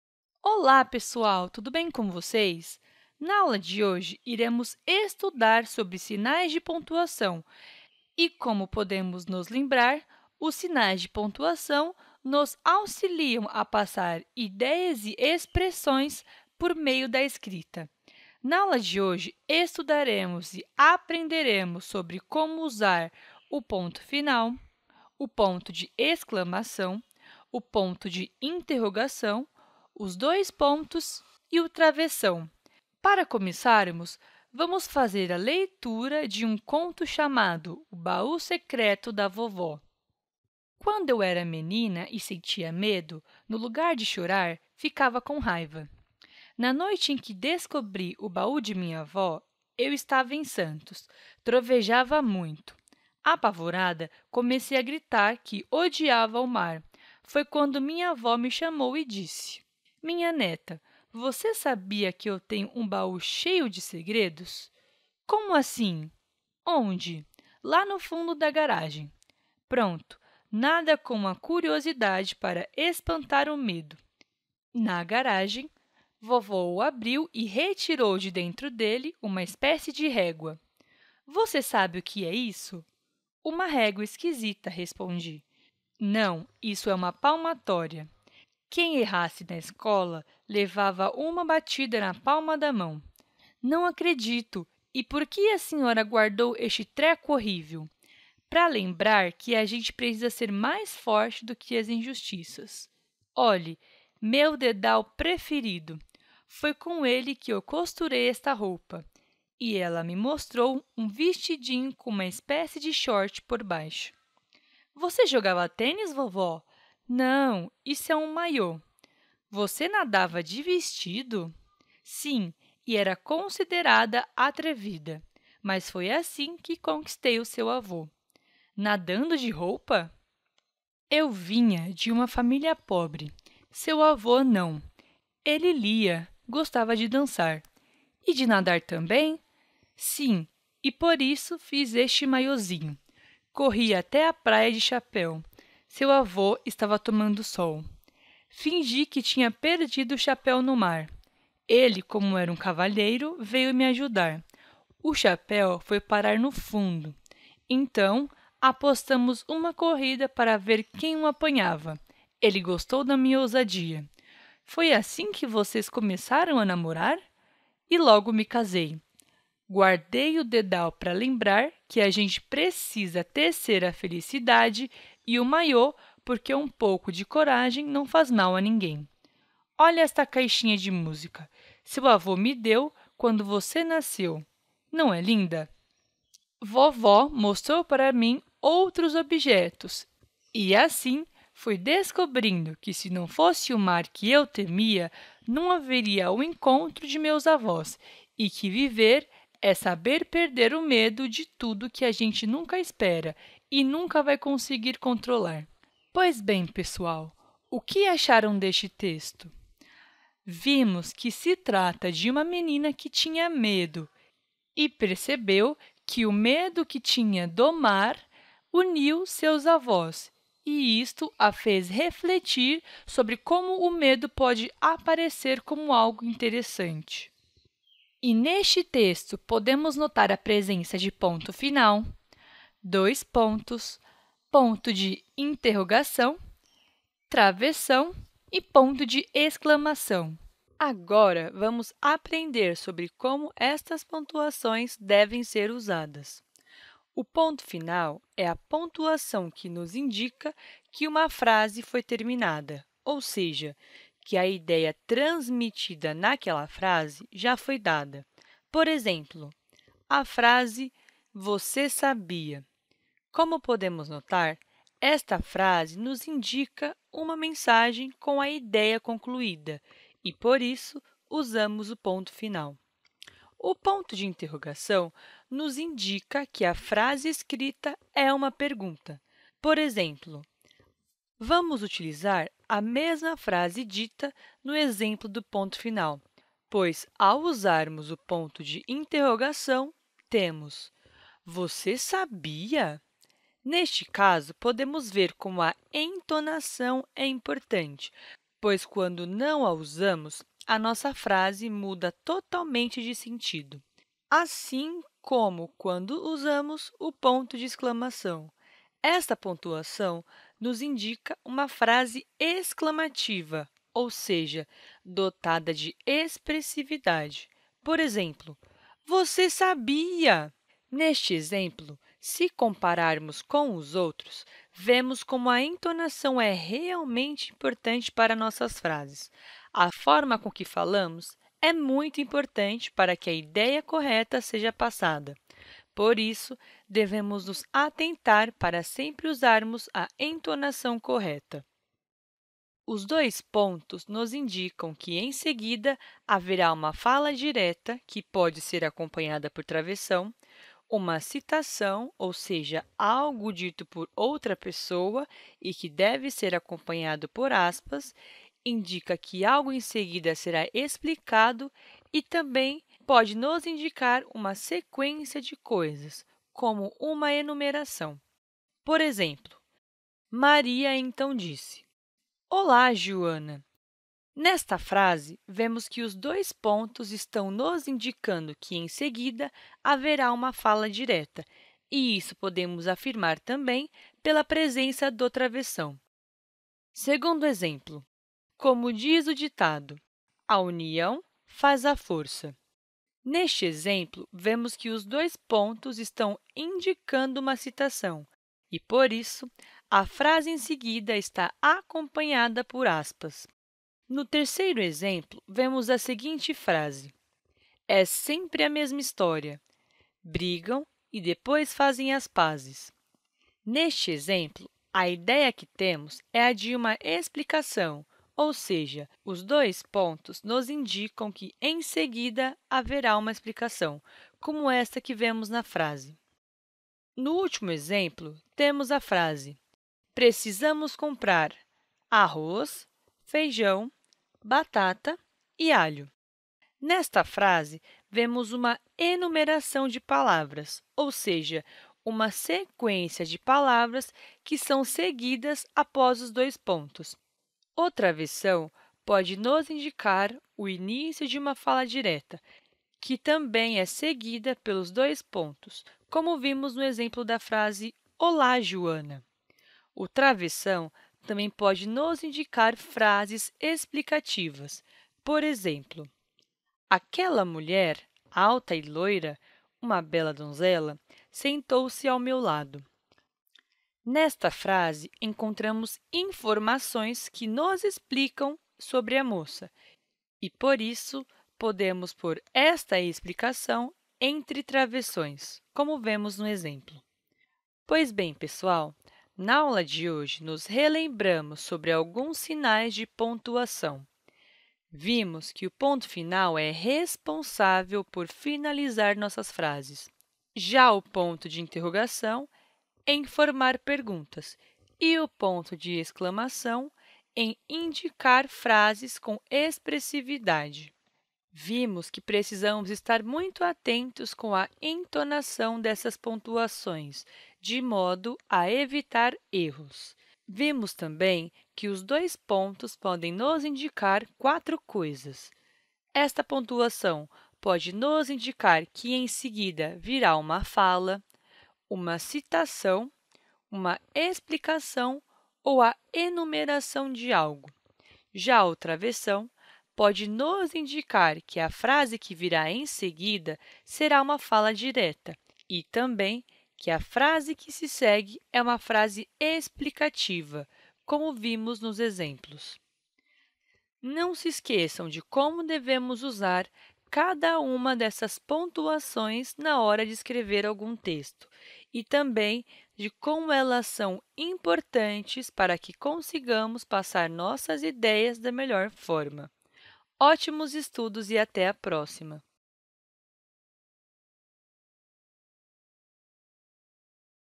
Como usar os sinais de pontuação?
Nesta videoaula, apresentamos informações sobre o uso de ponto final, interrogação, exclamação, dois-pontos e travessão. O aluno deve entender a função desses sinais e aprender a usá-los na escrita.